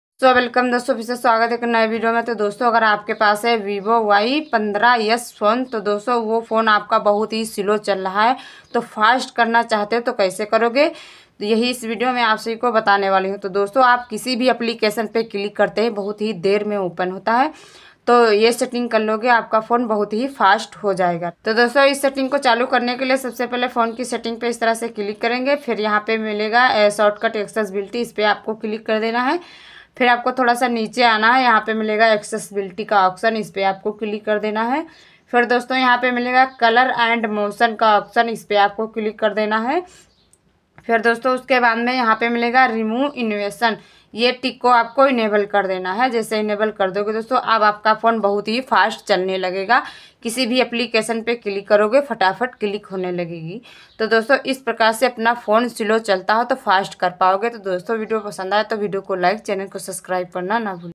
vivo y15s fingerprint screen lock setting | vivo y15s me fingerprint lock kaise lagaen
hello doston aaj ki is video mein humne aapko bataya Vivo y15s fingerprint lock Kaise lagaye

Vivo y15s#fingerprint#asha mobile tips